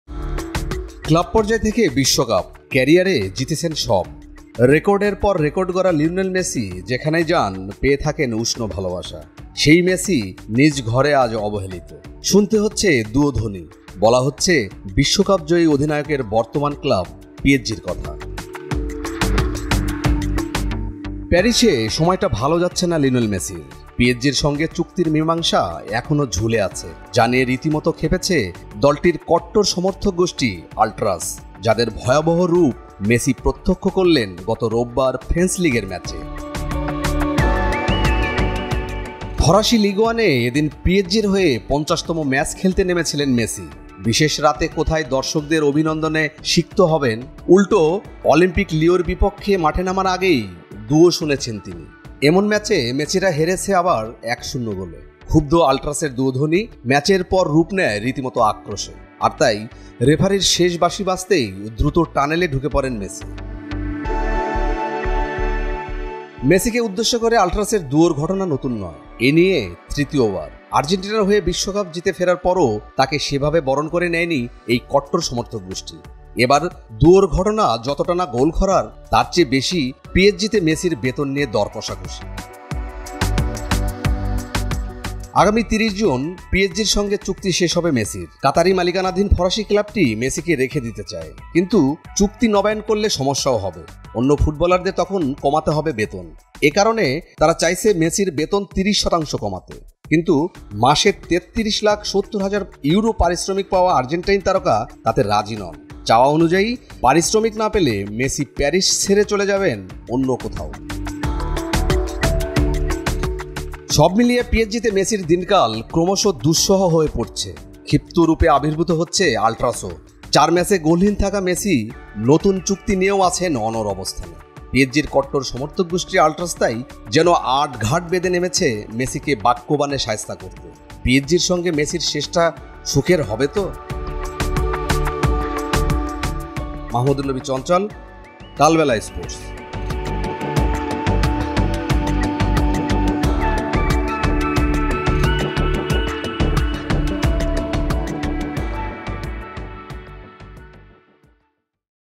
যে কারণে আলট্রাসের দুয়োধ্বনি শুনতে হলো মেসিকেও | Messi Haters | Kalbela Sports
ক্লাব পর্যায় থেকে বিশ্বকাপ। ক্যারিয়ারে জিতেছেন সব। রেকর্ডের পর রেকর্ড গড়া লিওনেল মেসি যেখানেই যান পেয়ে থাকেন উষ্ণ ভালোবাসা। সেই মেসি নিজ ঘরে আজ অবহেলিত। শুনতে হচ্ছে দুয়োধ্বনি। বলা হচ্ছে বিশ্বকাপজয়ী অধিনায়কের বর্তমান ক্লাব পিএসজির কথা।
That's why Messi also had to hear the echo of ultras | Messi Haters | Kalbela